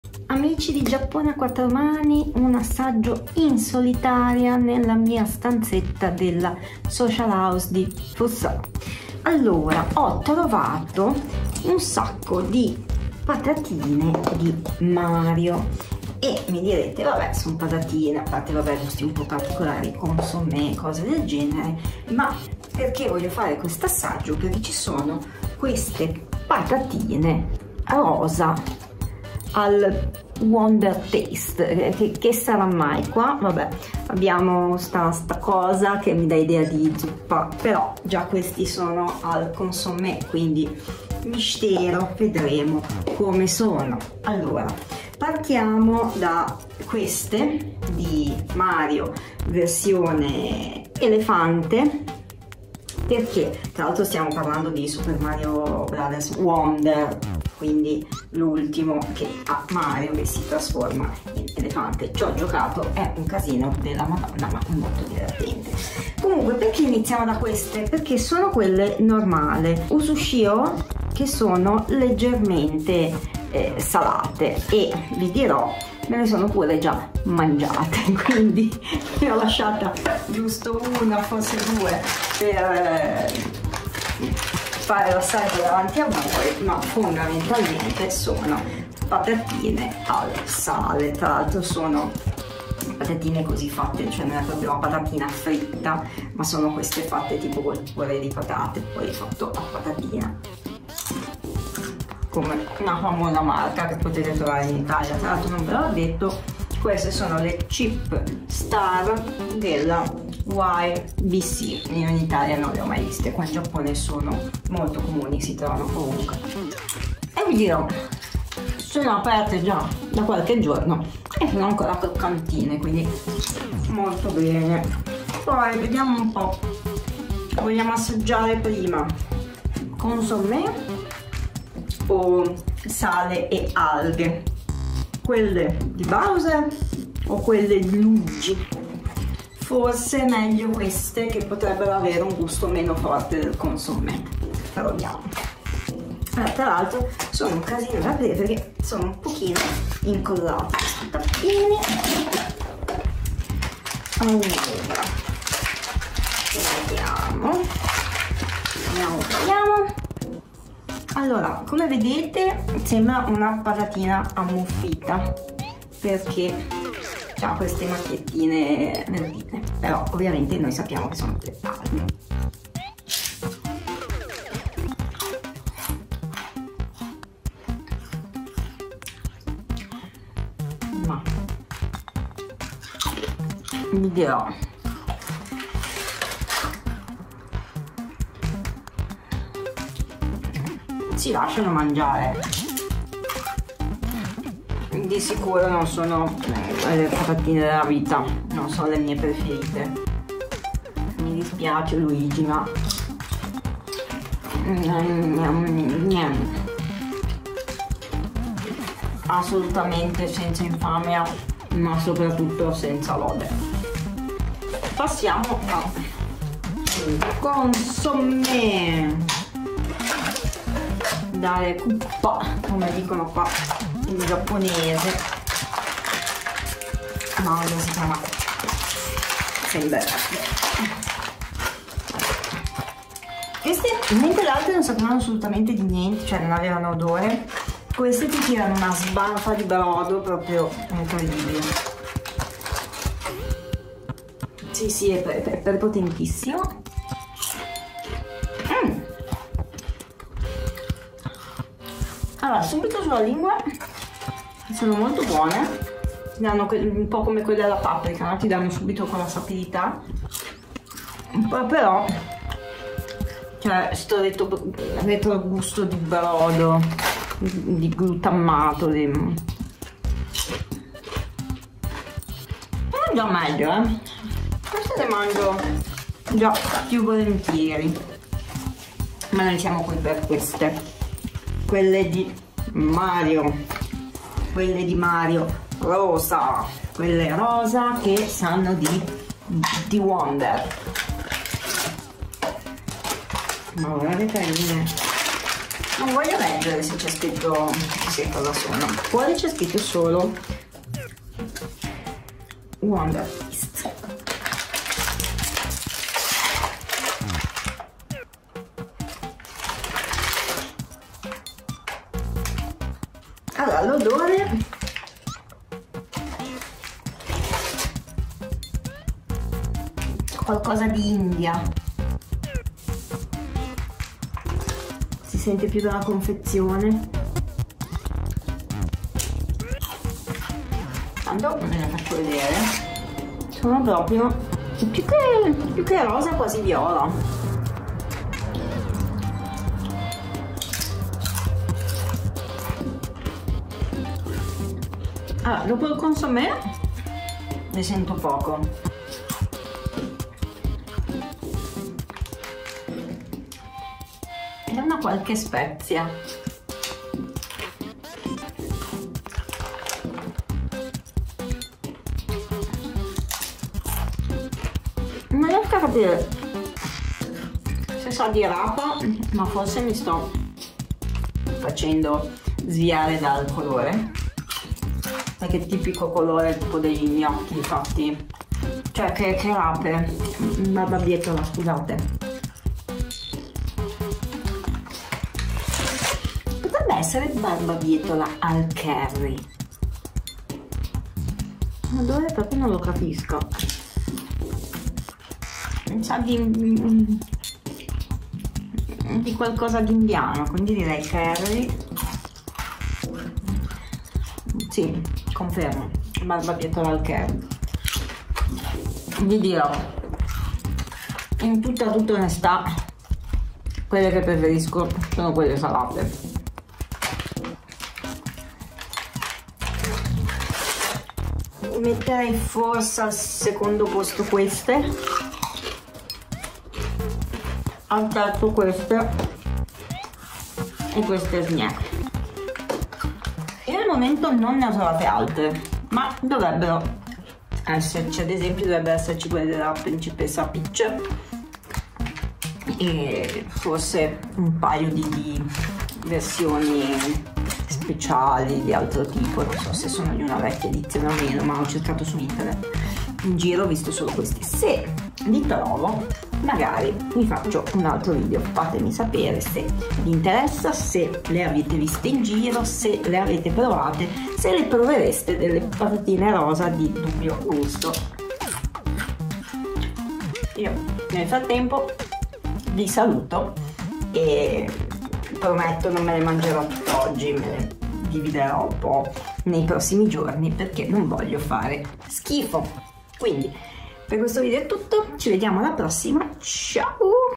Video assaggio: Patatine di Super Mario Bros
Assaggio per voi le patatine di Super Mario Bros. Quelle normali però le avevo già abbondantemente assaggiate in precedenza.

Ciò significa che se acquisti un prodotto/servizio attraverso uno di questi collegamenti, riceveremo una commissione, che in ogni caso non influisce sul prezzo del prodotto in quanto viene dedotta e non aggiunta al prezzo finale.